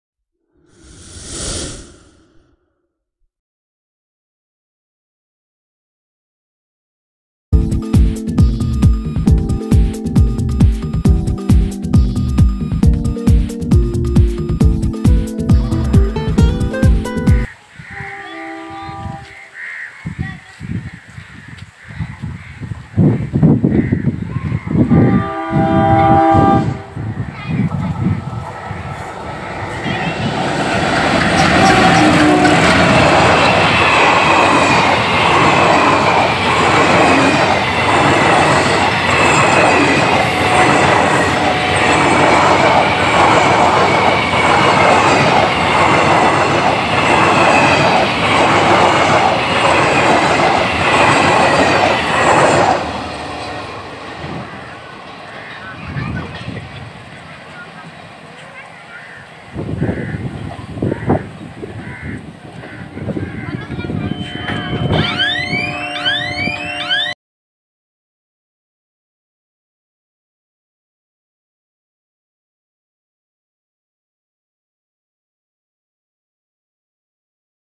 First Run After Lockdown 02261 Kolkata Haldibari Superfast Special Express with HWH WDM3D
02261 Kolkata Haldibari Superfast Special Express
First run After Lockdown Kolkata Haldibari Superfast express